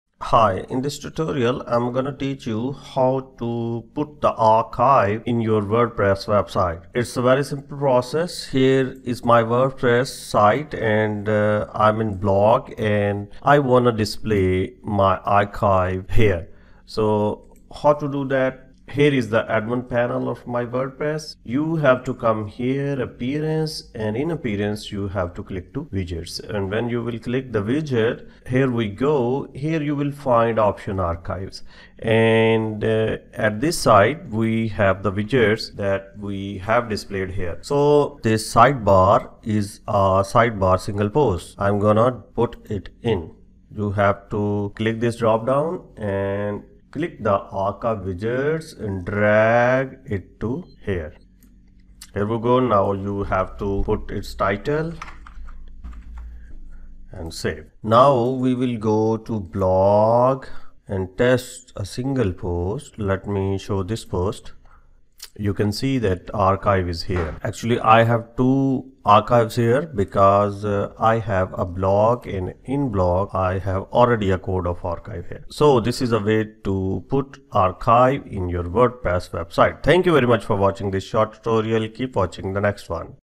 How to archive a page on wordpress ll Archive wordpress page 2023
Archiving a page on WordPress is a great way to keep your website clean and organized. In this video, I'll show you how to archive a page on WordPress.

Archiving a page on WordPress is simple and can be done in just a few minutes. After watching this video, you'll be able to archive a page on WordPress with ease!
In this video, we're going to show you how to archive a page on WordPress. Archive a page on WordPress allows you to keep a copy of a page online, so that you can reference it later.Archiving a page on WordPress is a great way to keep your web pages consistent and organised. If you need to save a copy of a page for later, archiving it on WordPress is the perfect solution! We'll walk you through the steps required to archive a page on WordPress, so that you can keep your pages consistent and ready for use!